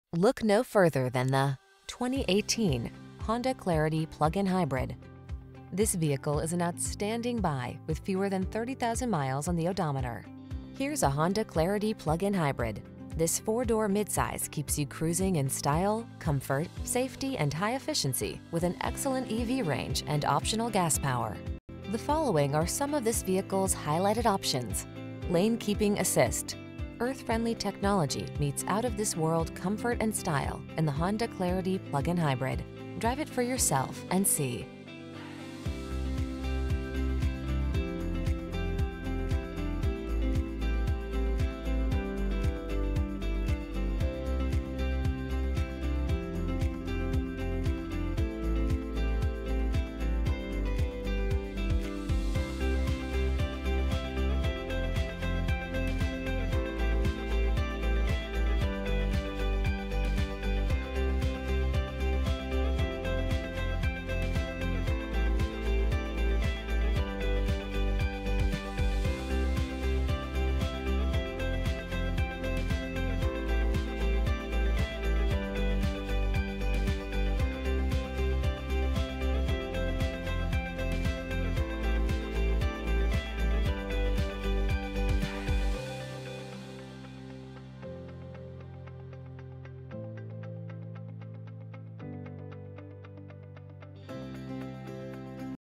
2018 Honda Clarity Plug-In Hybrid Gainesville, Newberry, Lake City, St. Augustine, Ocala, FL 10431
Solar Silver Metallic  2018 Honda Clarity Plug-In Hybrid available in Gainesville, Florida at Tomlinson Motor Co. Servicing the Gainesville, Newberry, Lake City, St. Augustine, Ocala, FL area.
2018 Honda Clarity Plug-In Hybrid Sedan - Stock#: 10431 - VIN#: JHMZC5F10JC004270